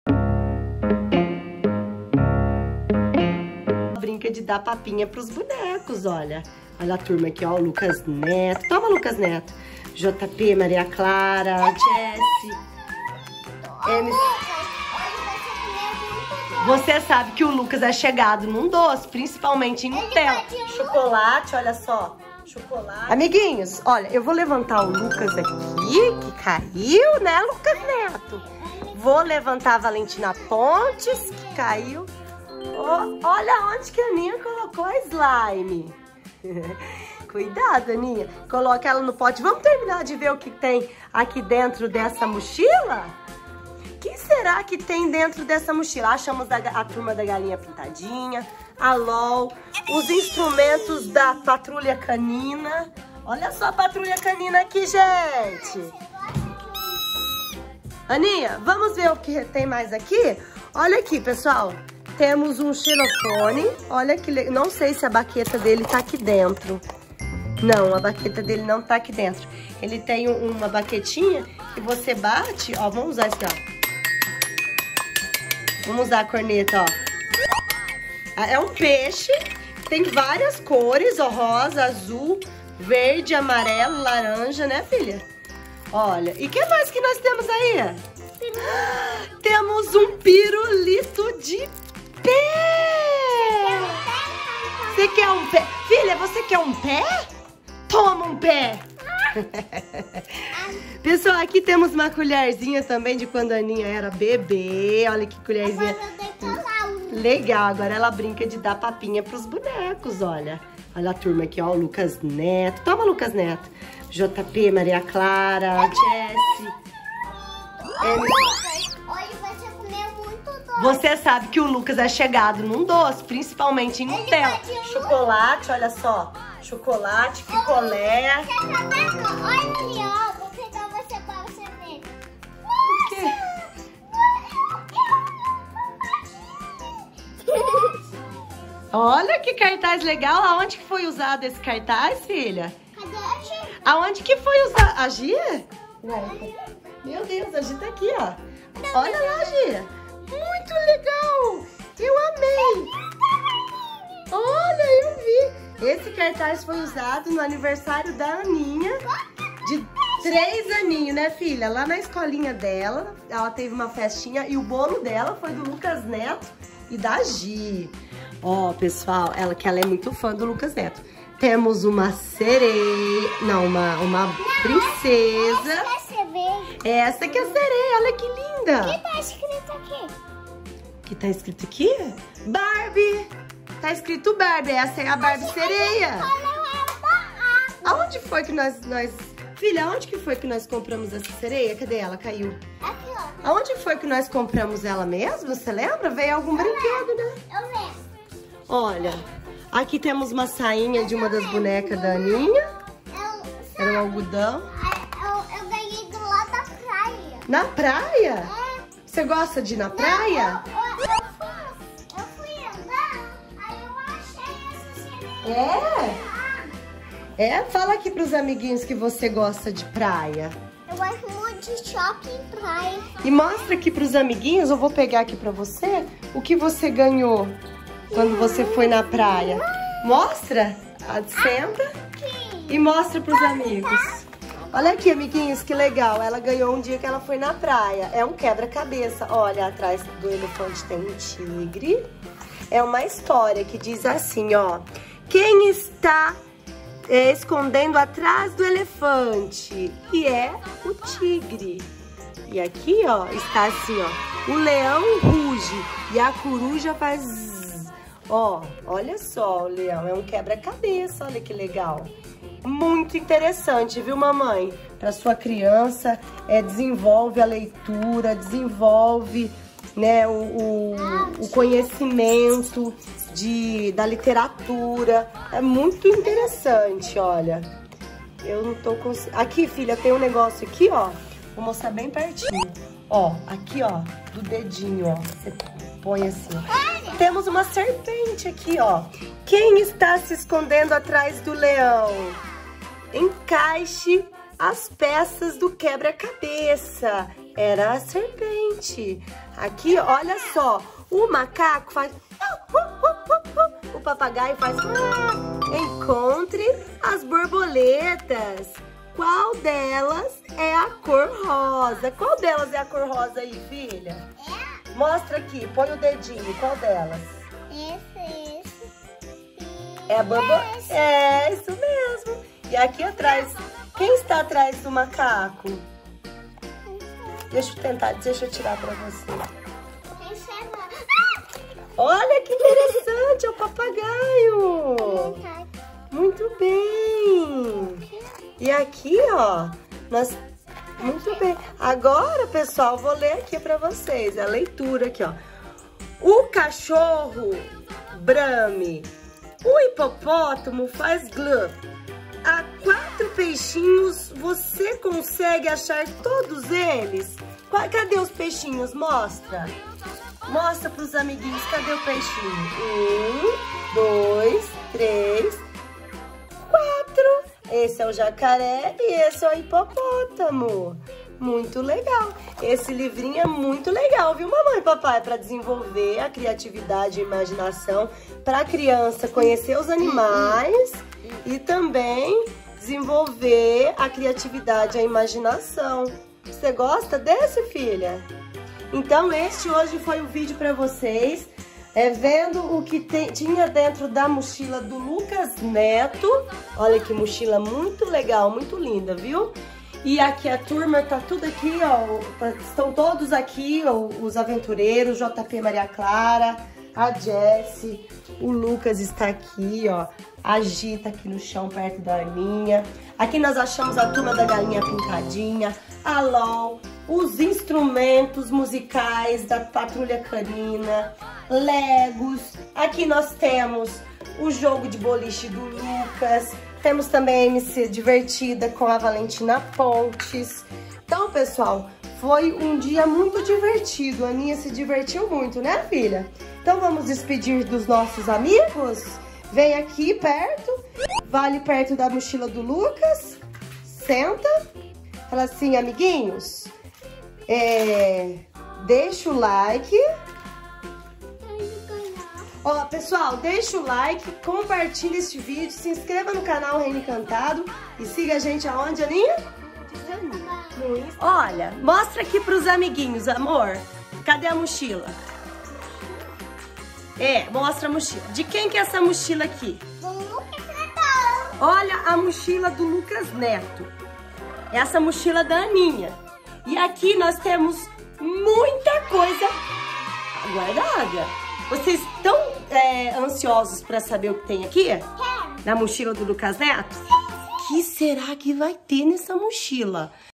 Luccas Neto Valentina Pontes Maria Clara JP Galinha Pintadinha Patrulha Canina [Bonecos e Brinquedo]
Bonecos Bonecas e Brinquedos. Anninha mostrando sua coleção, Luccas Neto Maria Clara JP Valentina Pontes MC Divertida aventureiros azul e vermelha Jessi Neto Patrulha Canina e muito mais...
✔️Canal Reino Encantado é livre para toda criança!